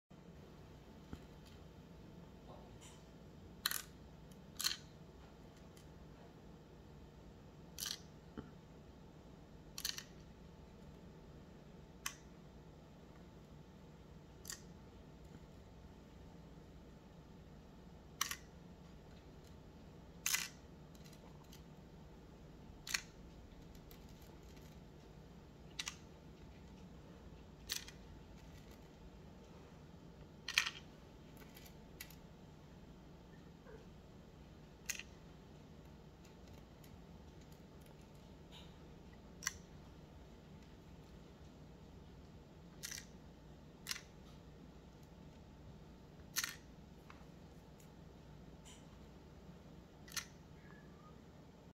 grounding rocks into balm tubes
Short video showing how I put little rocks into my grounding balm.